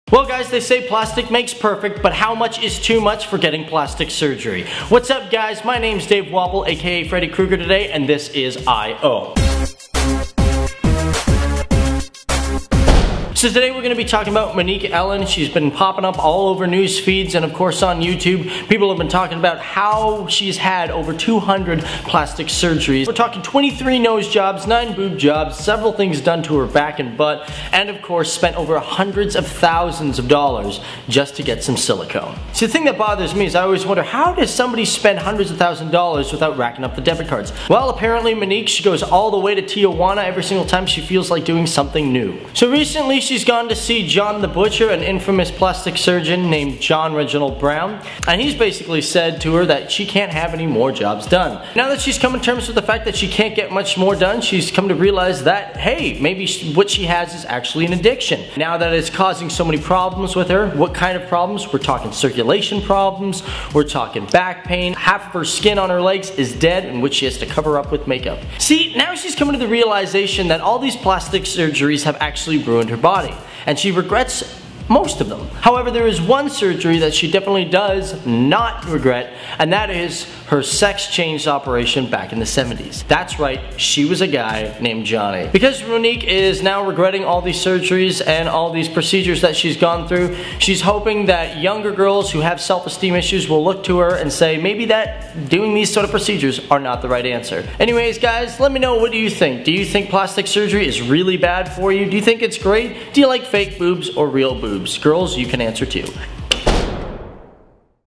Transgender Plastic Surgery Addict - Monique Allen
A surgery addict who was left disabled and disfigured after almost 100 unregulated cosmetic procedures now wants to speak out about the dangers facing unsuspecting women desperate for a quick fix.

Monique Allen, who has had more than 200 hundred plastic surgeries, became obsessed with pumping liquid plastic into her body and estimates she has injected almost 12 litres during 75 backstreet ops.

Over the years Monique, 58, has had a staggering 23 nose jobs and nine breast augmentations - taking her to size 38FFF.